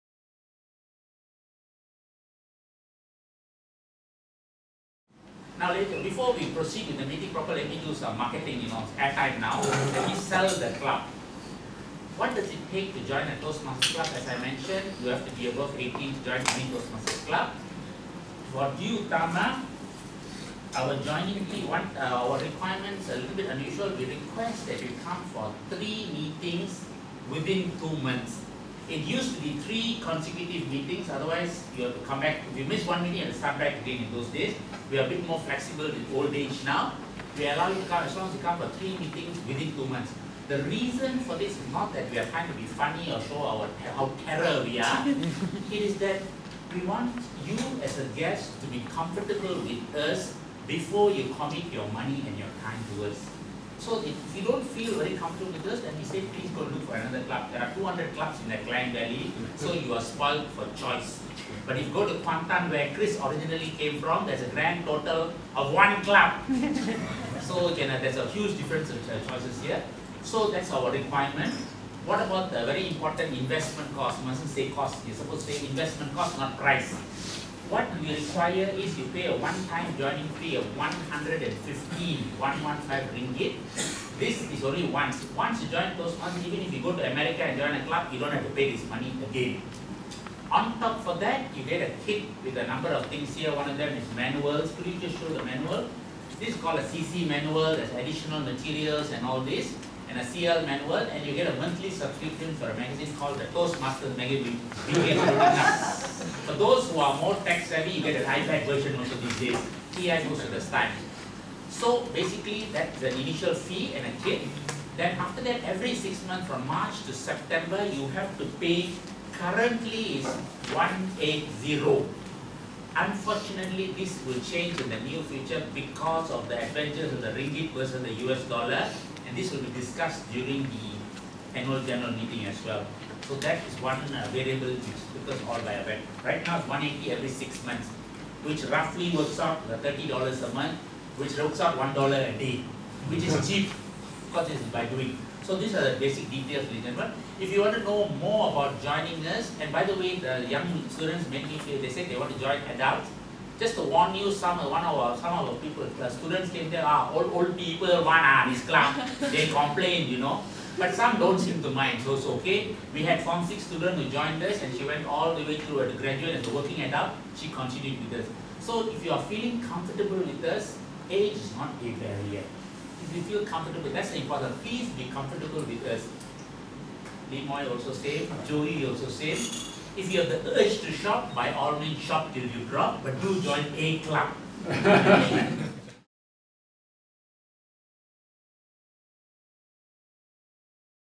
D'Utama Toastmasters Club Membership
D'Utama Toastmasters Club (#5912): D'Utama Toastmasters Club Membership.

Date:  7 April 2016 (Thursday)
Venue: Room 1.05, Kolej Damansara Utama (KDU)

Recorded with Sony FDR-AXP35 with 4K resolution (Ultra High Definition).